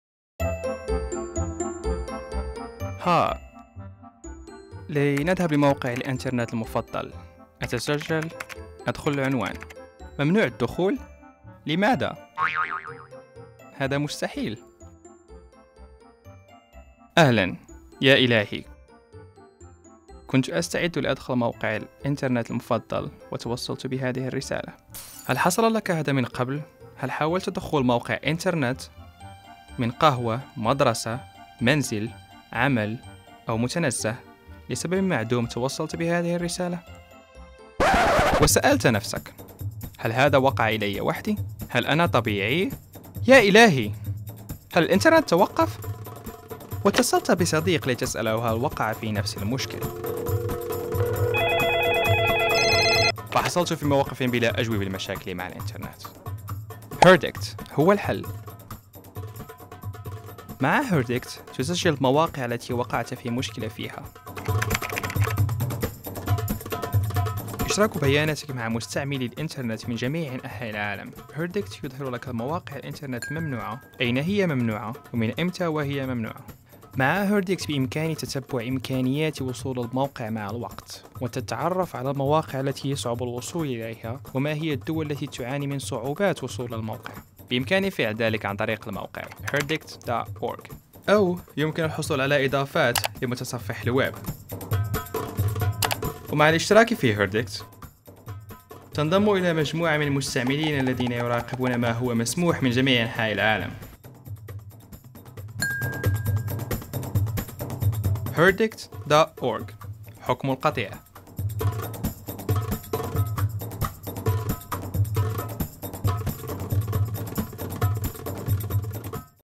ما هو Herdict؟
الخراف كحيل يدل Herdict ، مذهلة موقع جديد لمساعدة المستخدمين على استعادة الشبكة. هل لديك مشاكل مع الشبكة أو ترشيح المواقع التي يتعذر الوصول إليها؟ اختبار Herdict في